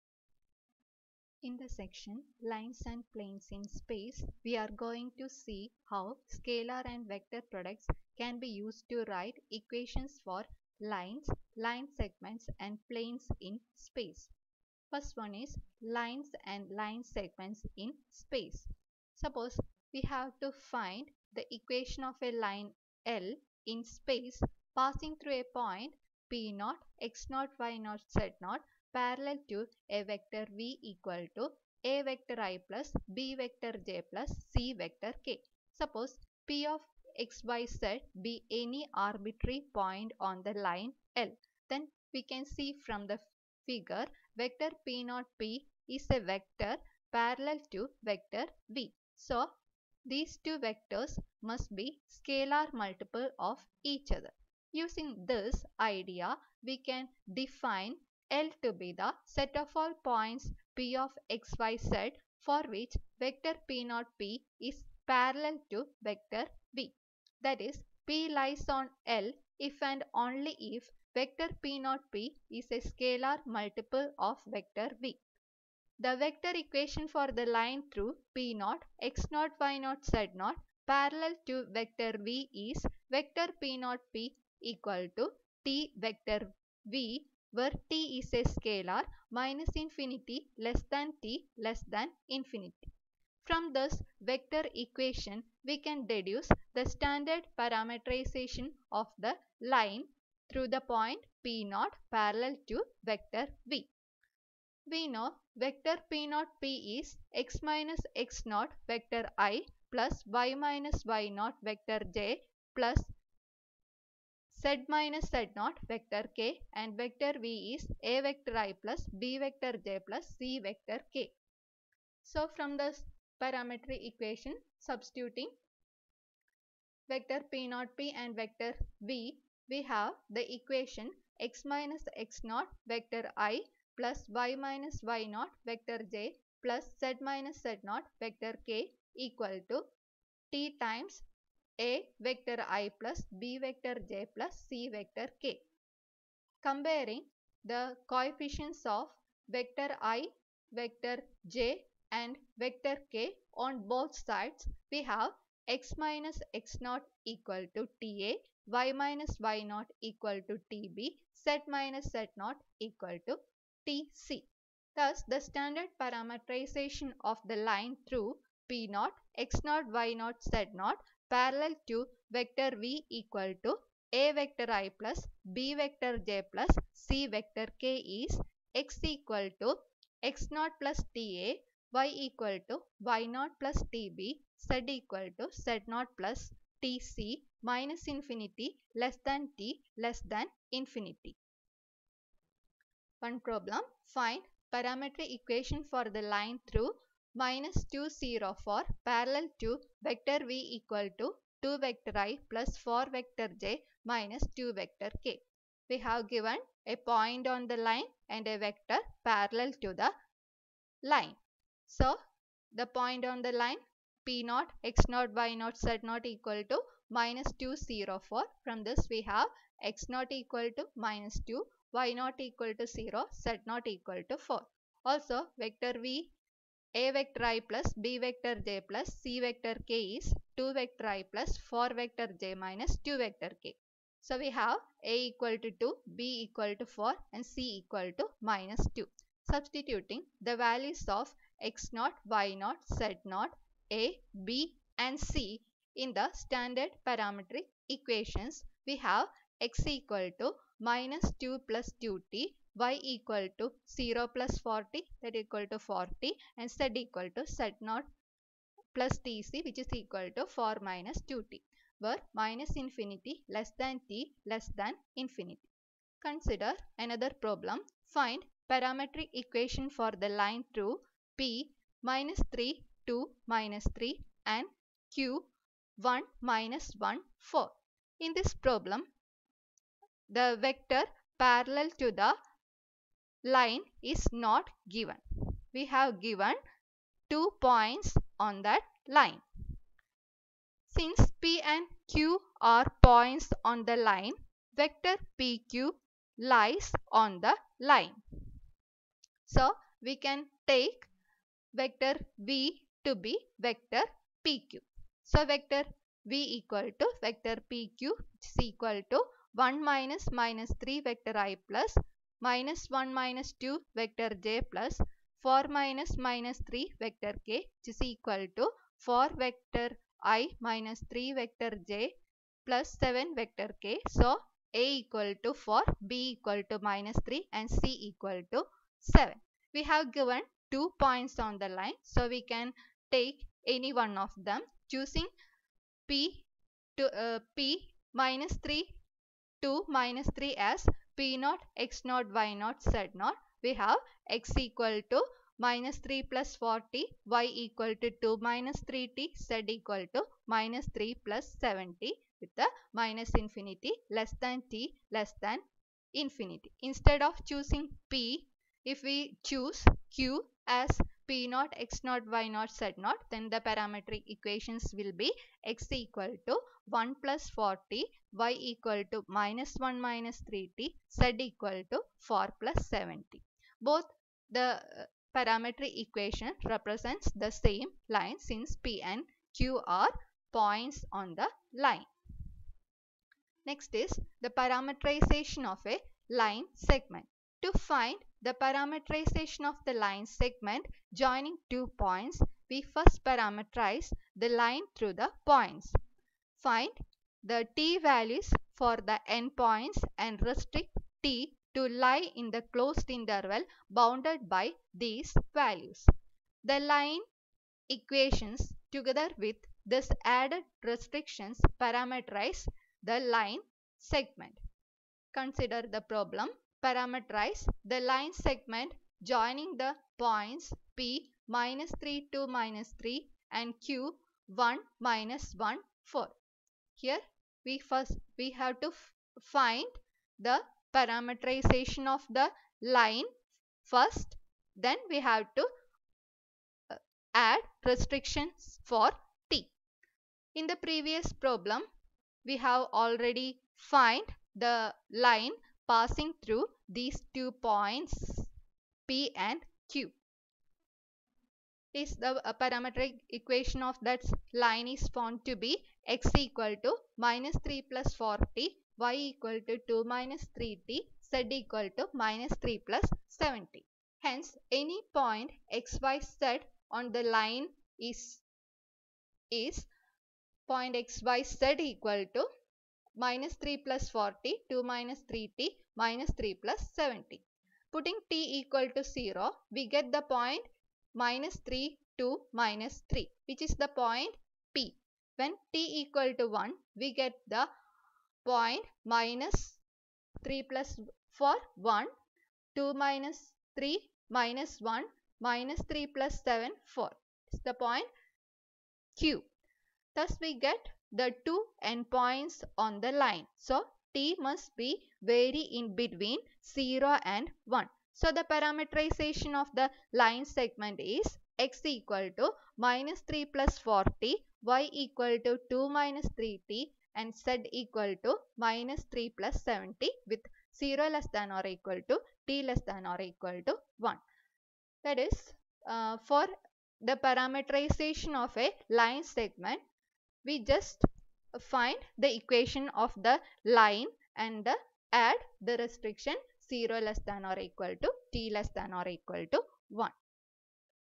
MAT4B04 -Session6- Part 2-Deepthi A N, Assistant Professor, Sree Narayana College, Nattika
Lines and Planes in Space